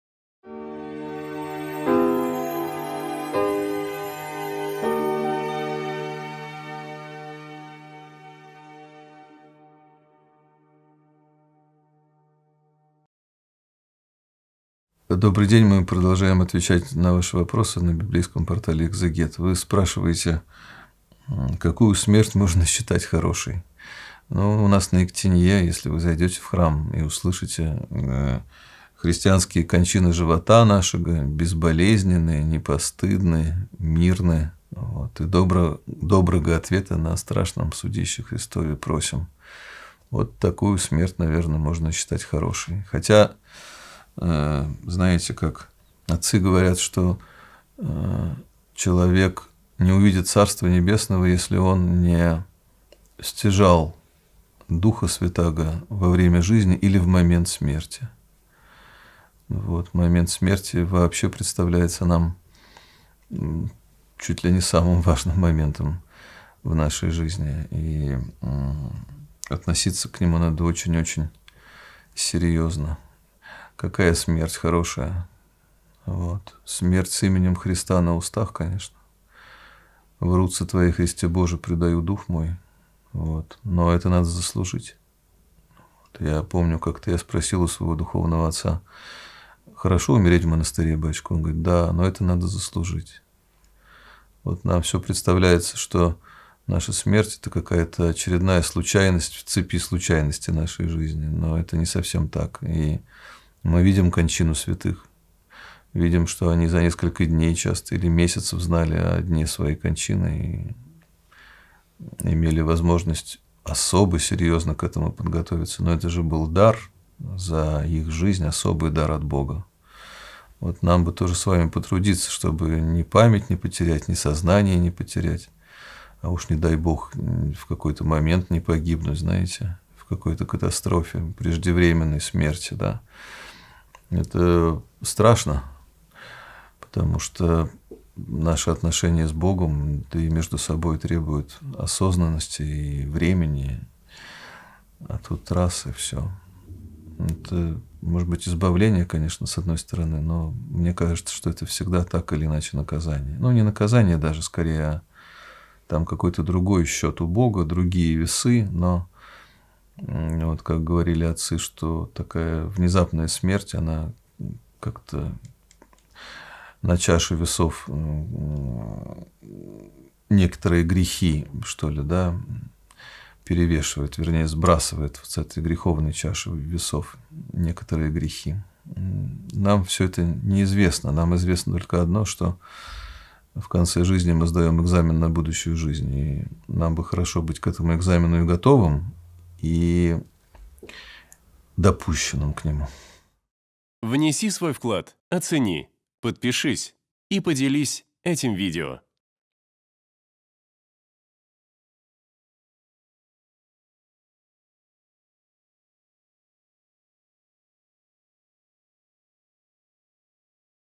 КАКУЮ СМЕРТЬ МОЖНО СЧИТАТЬ ХОРОШЕЙ ? Протоиерей Дмитрий Рощин
Команда Экзегет.

В сегодняшнем видео на библейском портале Экзегет вместе со священником Димитрием Рощиным отвечаем на вопрос: "Какую смерть можно считать хорошей?"

🔸  Стяжание Духа Святого
🔸  Подготовка к смерти
🔸  Будущая жизнь